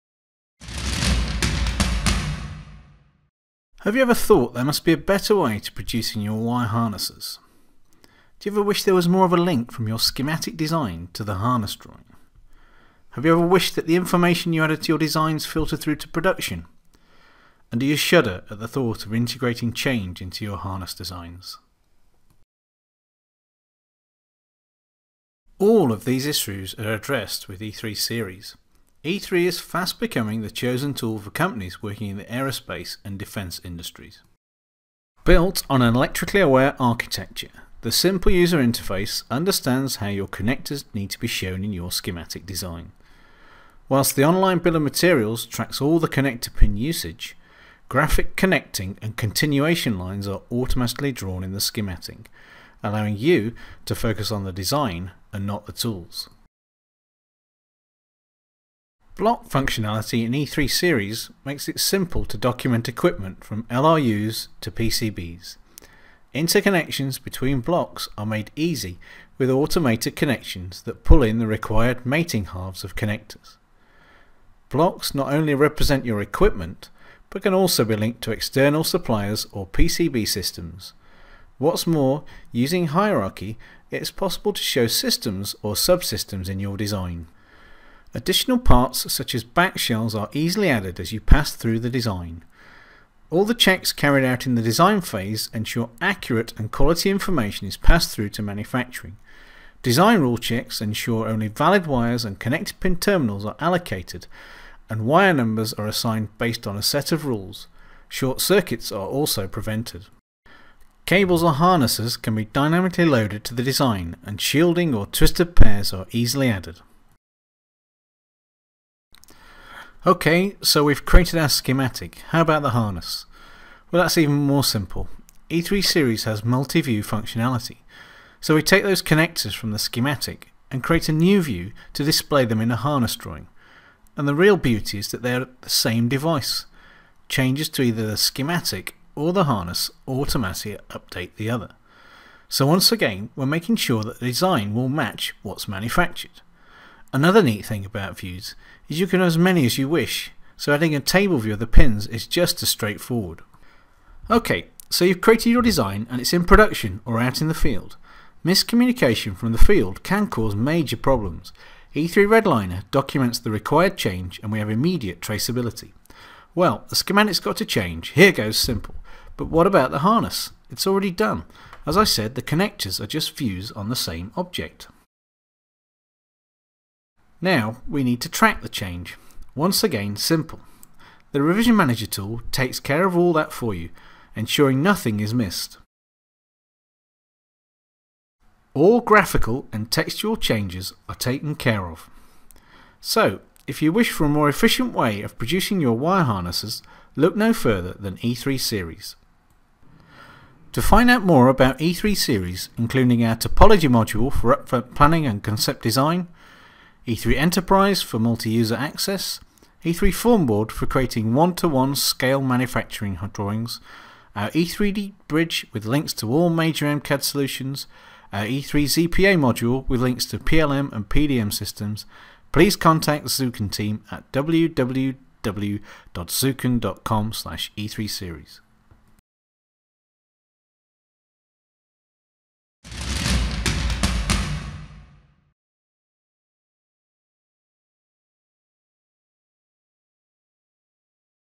E3.series: Aerospace and Defense Industry Wire Harness and Control System Design
- Have you ever thought there must be a better way to produce your wire harnesses? Ever wish there was more of a link from your schematic design to your harness drawing? Do you wish that the data you add to your designs filtered through to production? Does introducing change into your harness designs cause you to shudder?...
All of these issues and more are addressed with E3.series from Zuken.